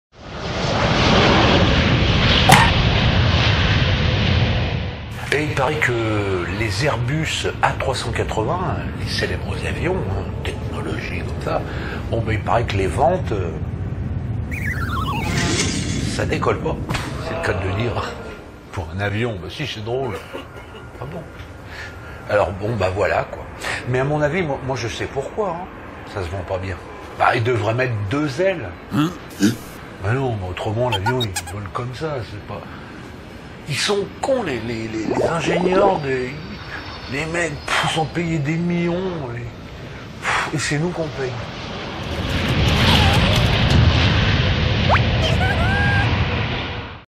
Les Airbus A380 se vendent mal - Pourquoi ?
Les Airbus A380 ne se vendraient pas (parait-il?) J'ai donc mené mon enquête !!